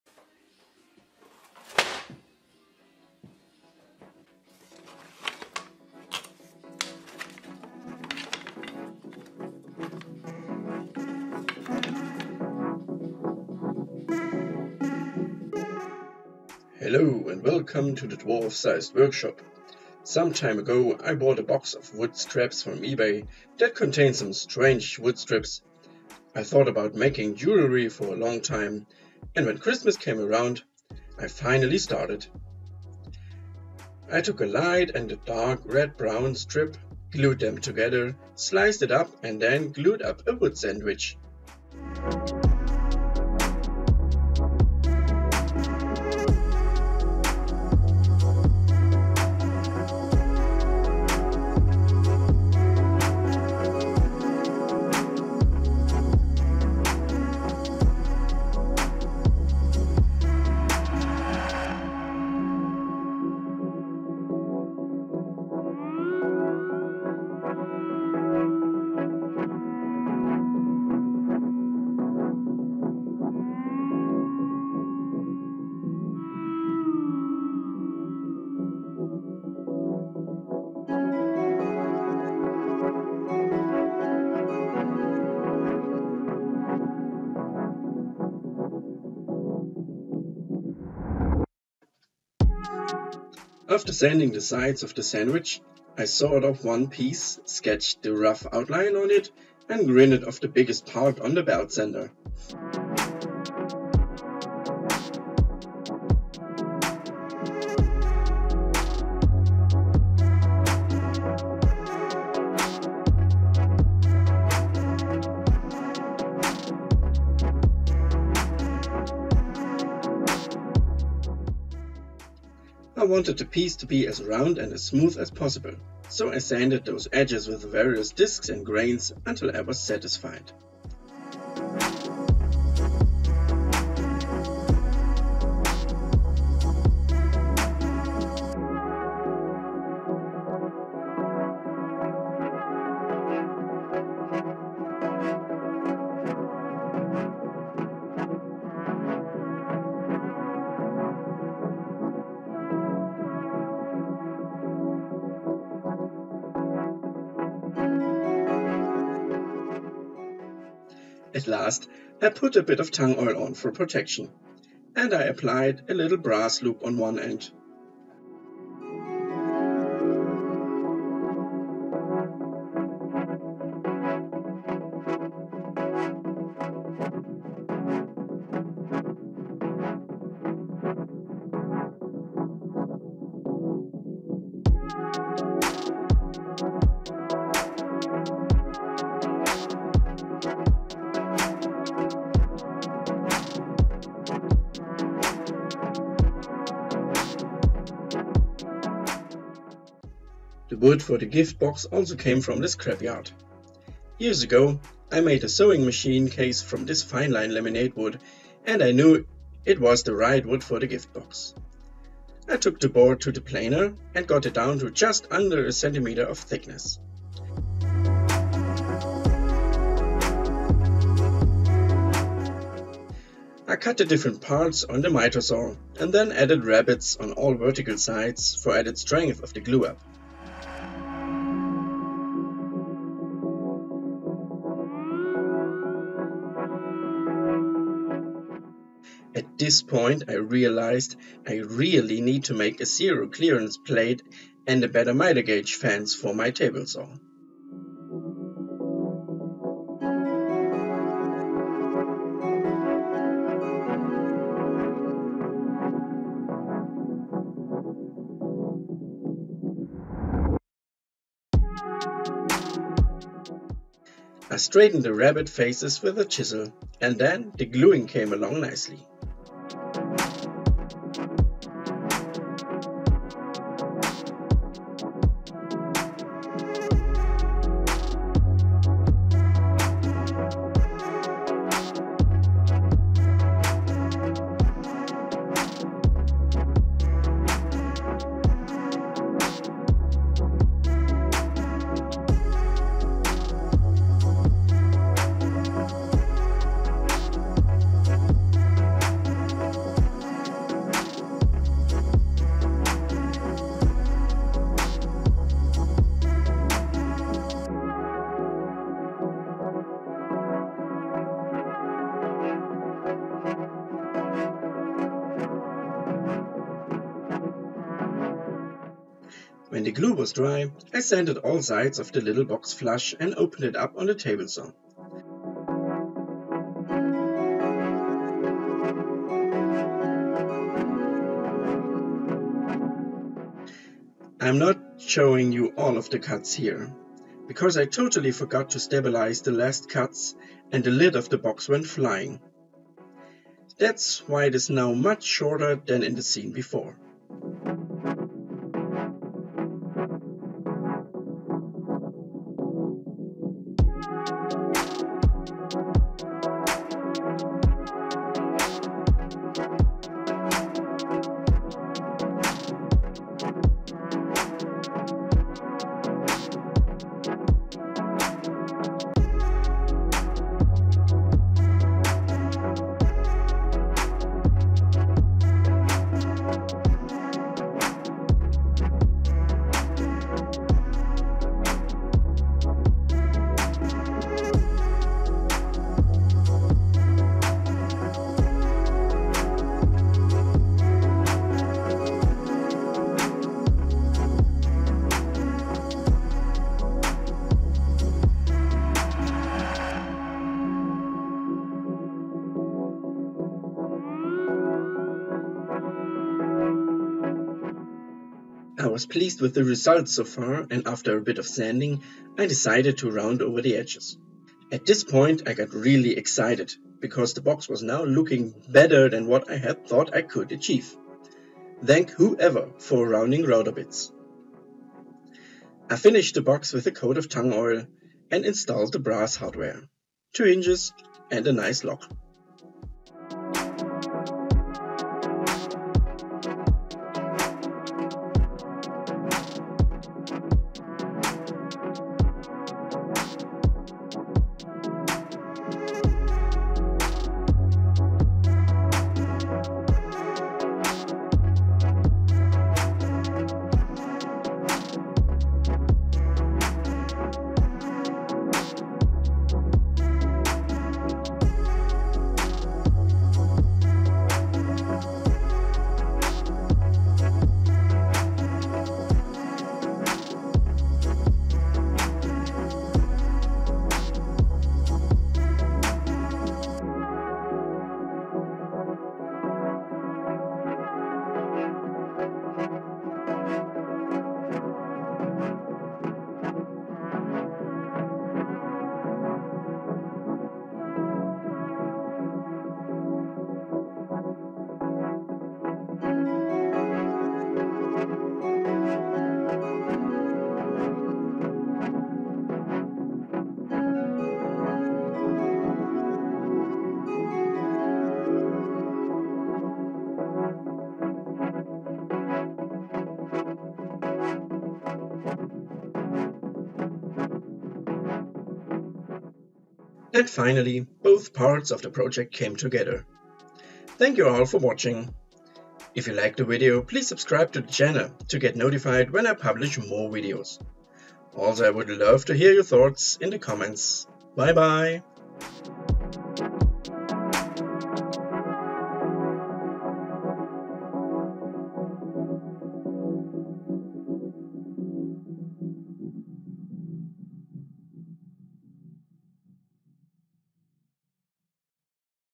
Making Wooden Jewelry out of Scrap Wood
Some time ago, I bought a box of scraps from eBay that contains some strange wood strips. I thought about making jewelry for a long time and when christmas came around, I finally started.